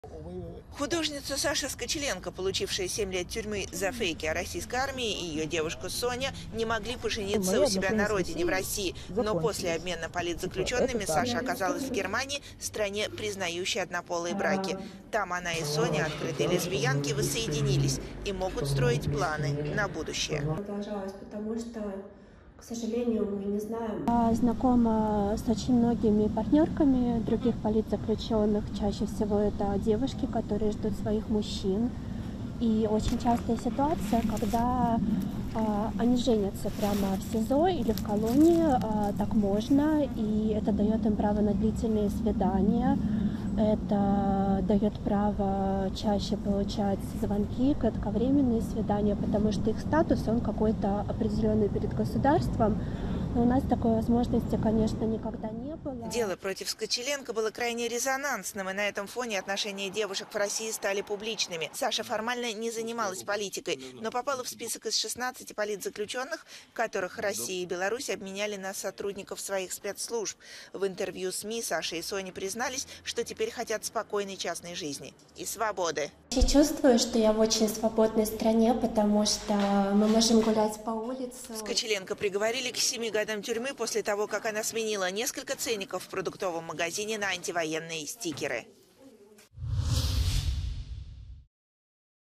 Саша Скочиленко: новая жизнь в Германии
Саша Скочиленко, художница и музыкант, была освобождена из российской тюрьмы в рамках недавней процедуры обмена политзаключенных. Теперь она и ее девушка Соня осели в Германии, где мечтают о спокойной частной жизни  и не исключат возможности пожениться.